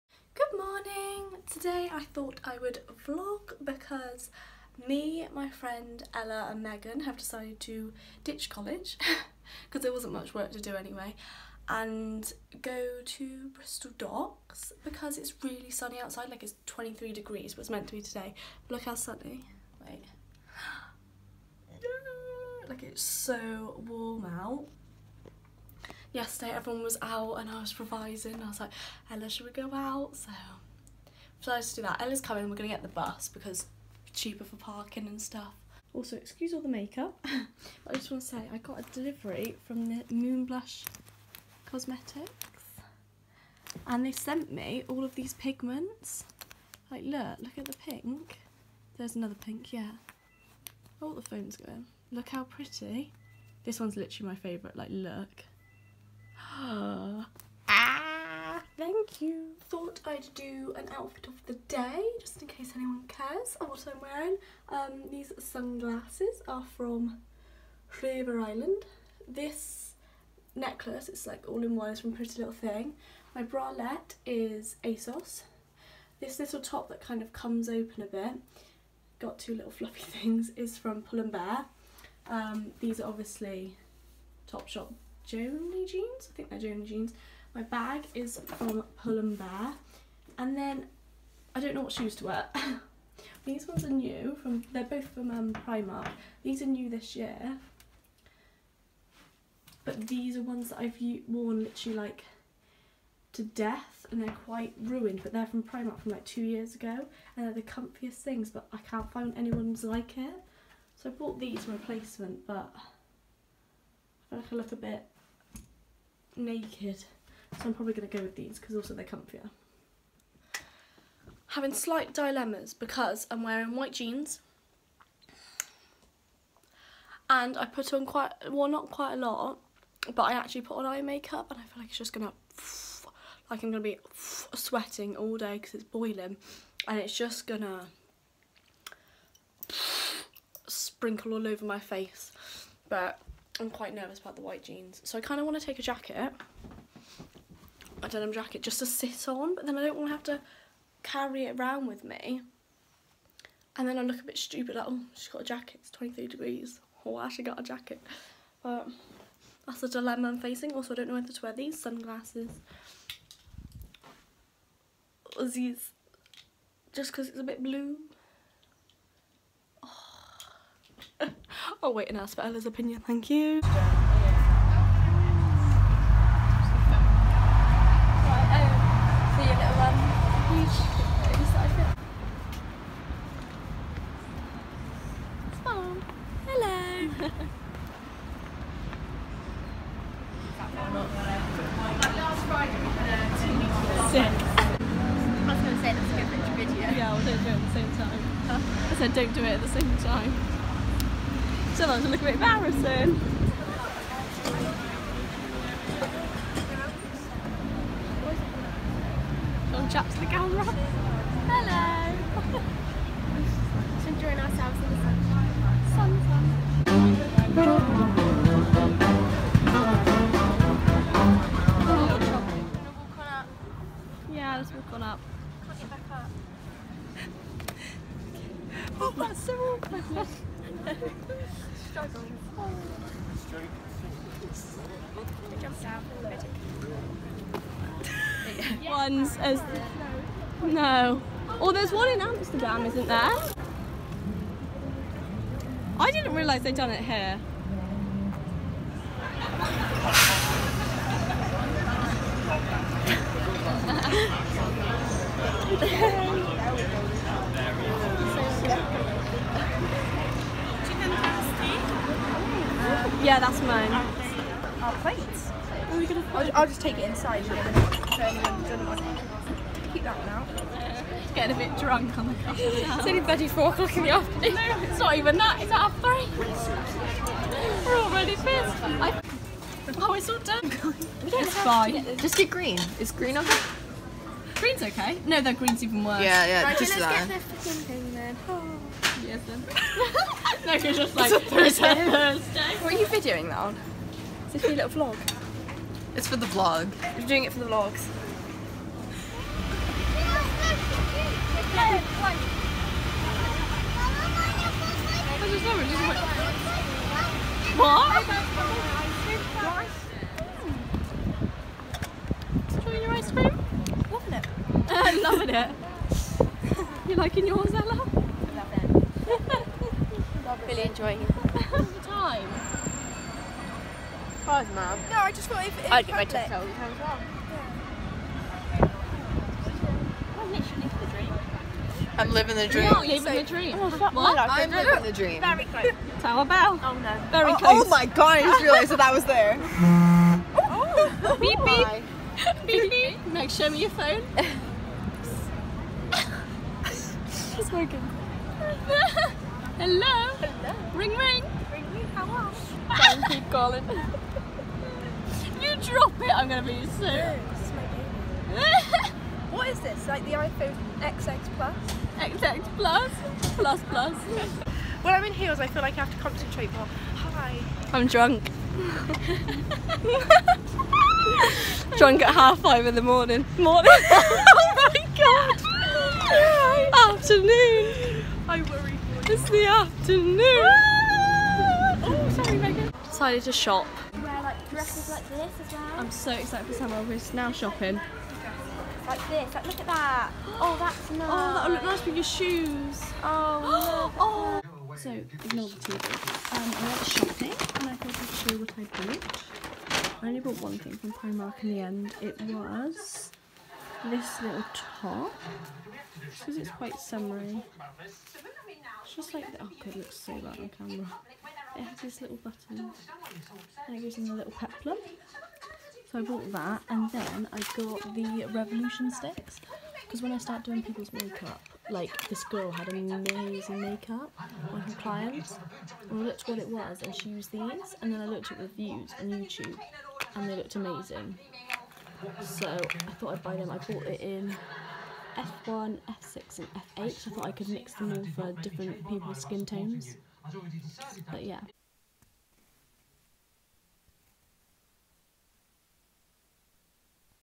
COCKTAILS, DRUNKNESS & SUNSHINE | Sophie Cooke ♡
This vlog was very short as I felt nervous picking up the camera in public especially as it was super busy, was such a lovely day and just thought I’d record a bit of it, we didn’t do much apart from drink and eat. Xx